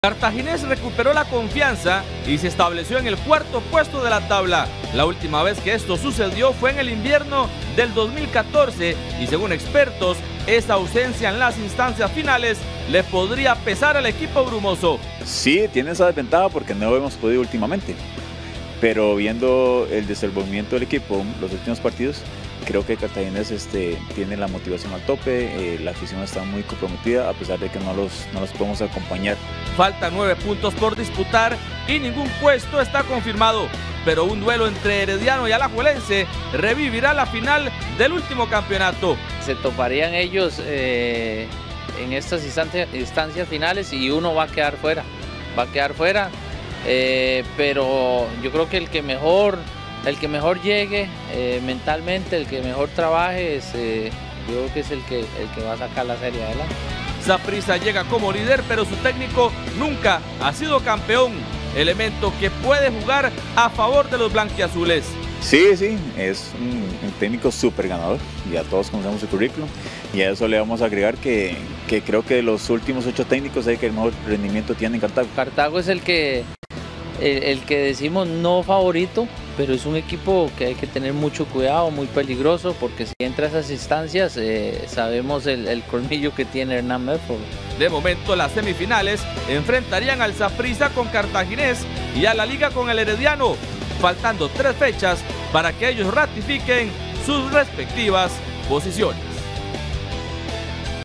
Semifinales se jugarían con los 4 grandes
Saprissa y Alajuelense son los favoritos por su rendimiento en el torneo.

Los ex jugadores de los grandes analizan lo que serían las semifinales faltando tres fechas para que los equipos ratifiquen sus posiciones.